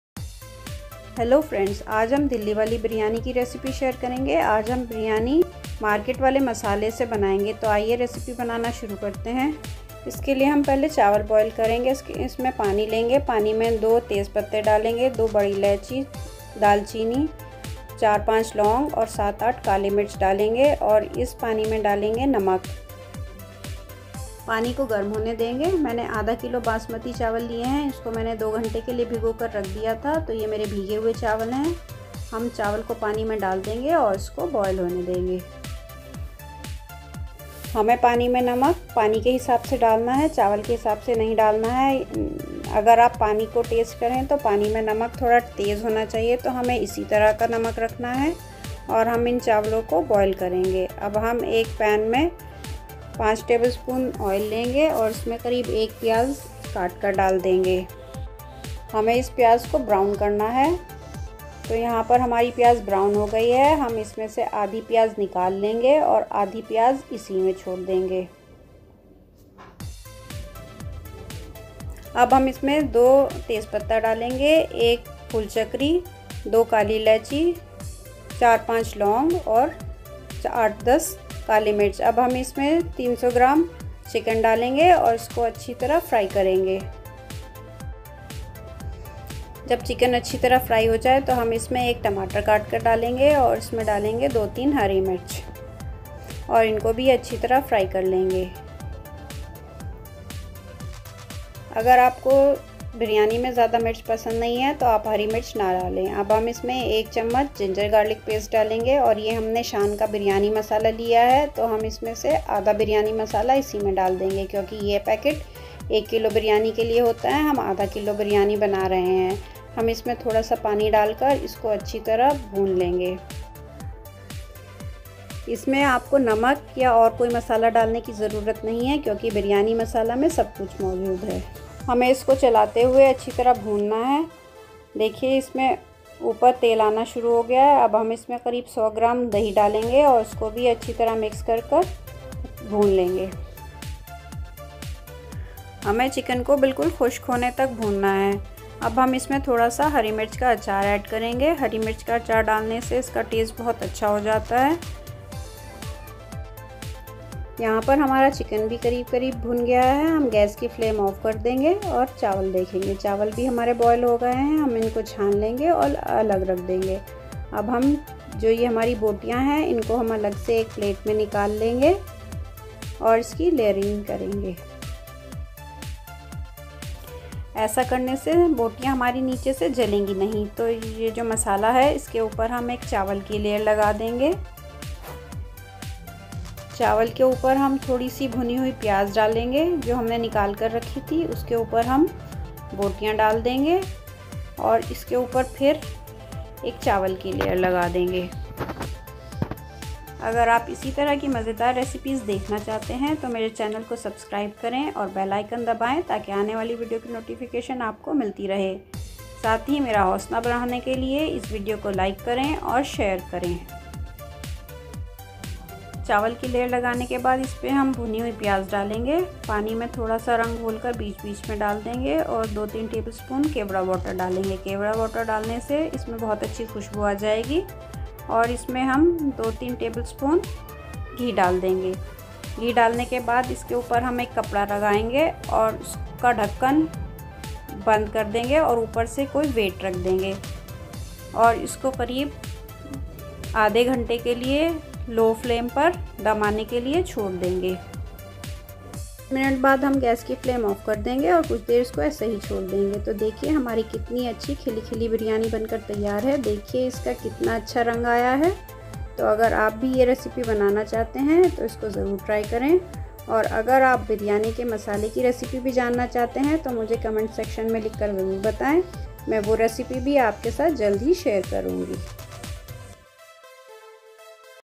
jhatpat banaye dilli6 wali biryani ghar par/ masala chicken biryani
ingredients
basmati rice 500gms
chicken 300gms
shan biryani masala
curd 100 gms
ginger garlic paste 1teaspoon
tomato 1
green chilli 3
onion 1
orange food colour
kevda water 3tbsp
oil5 tbs
bay leaves, green cardamom, black cardamom, cloves, black pepper, staranis
ghee 2tbsp

pls watch the video for recipe

cook with UB